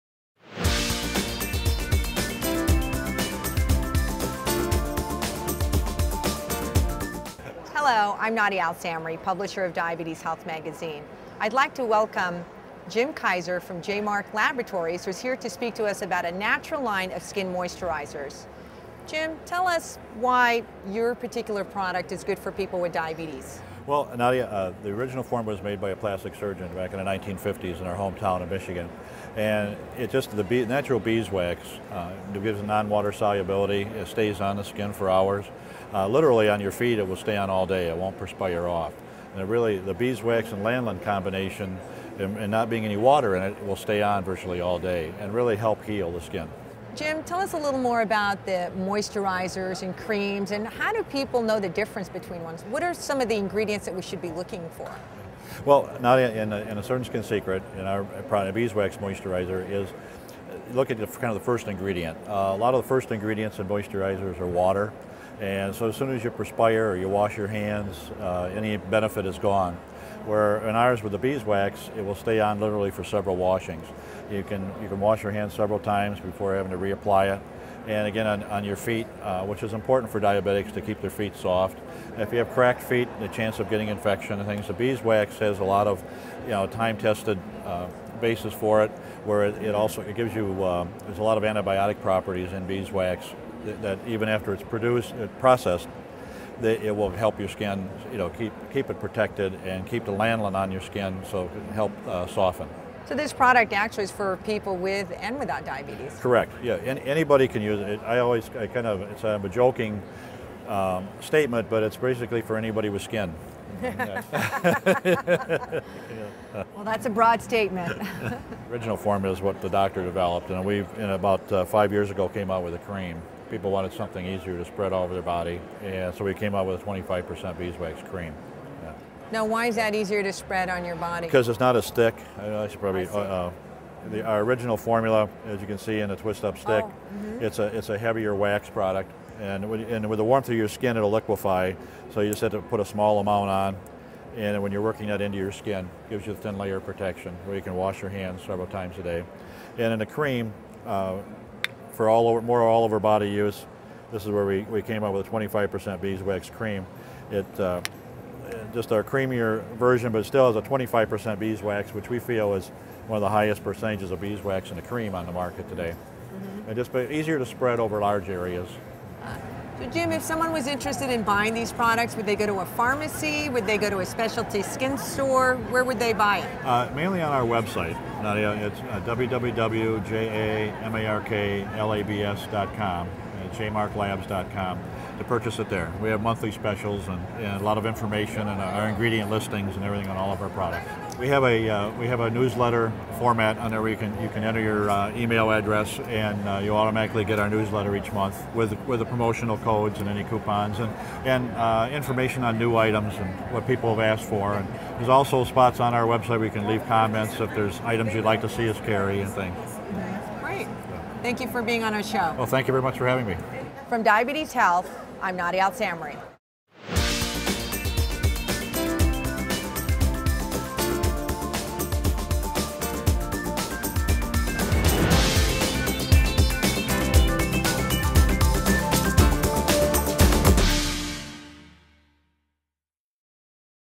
Surgeon's Skin Secret
A beeswax and lanolin skin moisturizer originaly formulated to treat surgical scars is now helping diabetes patients take better care of their skin.